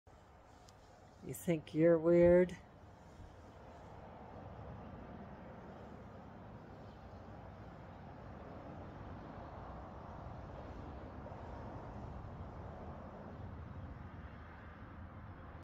And you think you're weird?
Too long in the sun, having too much fun. I had a wait in a ski lodge, poor me, and I said this to a passersby.